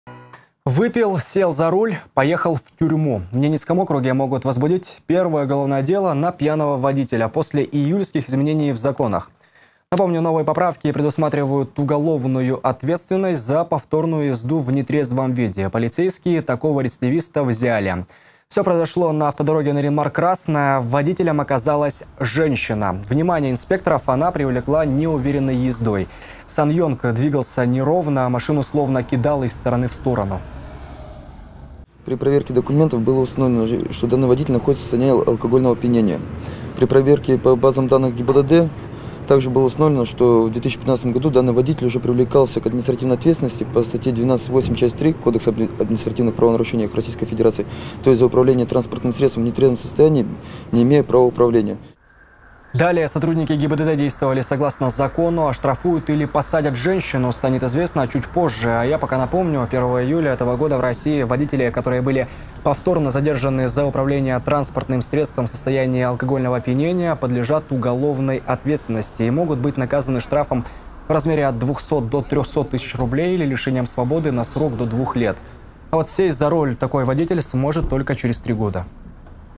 В НАО водителю-рецидивисту грозит срок за пьяное вождение
Выпил, сел за руль — поехал в тюрьму! В Ненецком округе могут возбудить первое уголовное дело на пьяного водителя после июльских изменений в законах. Напомним, новые поправки предусматривают уголовную ответственность за повторную езду в нетрезвом виде. Полицейские НАО такого рецидивиста уже взяли.

Все произошло на автодороге Нарьян-Мар — Красное. Водителем оказалась женщина! Внимание инспекторов она привлекла неуверенной ездой. SsangYong двигался неровно, машину словно кидало из стороны в сторону.

— При проверке документов было установлено, что данный водитель находится в состоянии алкогольного опьянения, — сообщил  и.о. начальника ОГИБДД УМВД России по НАО Александр Павлинов. — По базам данных также было установлено, что в 2015 году данный водитель уже привлекался к административной ответственности за управление транспортным средством в нетрезвом состоянии, не имея права управления.

Оштрафуют или посадят женщину, станет известно чуть позже. Напомним, с 1 июля этого года в России водители, которые были повторно задержаны за управление транспортным средством в состоянии алкогольного опьянения, подлежат уголовной ответственности и могут быть наказаны штрафом в размере от 200 до 300 тысяч рублей или лишением свободы на срок до 2 лет. А вот сесть за руль такой водитель сможет только через 3 года.